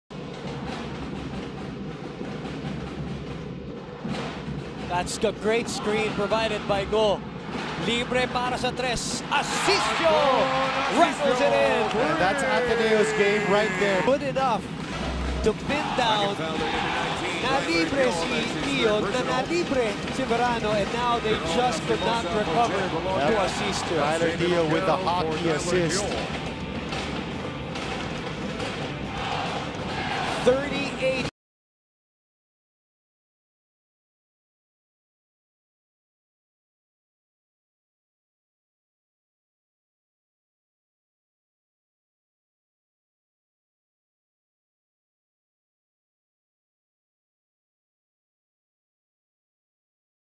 UAAP 81 MB: Beautiful Ateneo passing leads to Anton Asistio three | ADMU | Best Plays
Tyler Tio finds Raffy Verano who then finds Anton Asistio open for the splash!